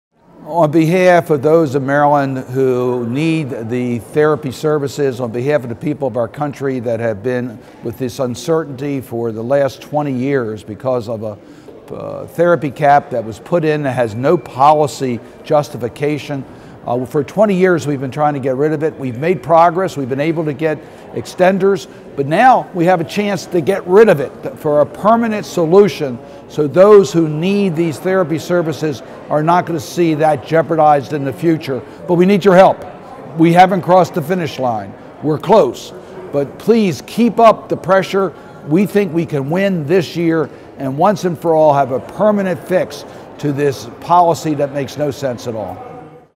Senator Cardin re Therapy Caps 11 29 17
Therapy caps have been harming patients for 20 years. It's time to StopTheCap. Our thanks to @SenatorCardin for his leadership and advocacy toward a permanent solution!